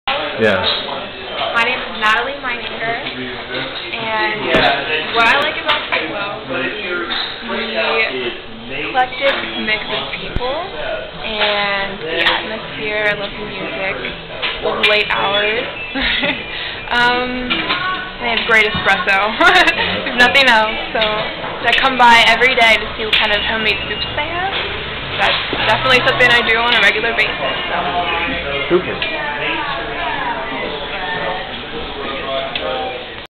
Natalie loves Sitwell's Coffee House
Natalie likes Sitwells because of the eclectic people, late hours and great espresso. She comes by nearly every day to see what kind of homemade soups they have. It is definitely something she does on a regular basis.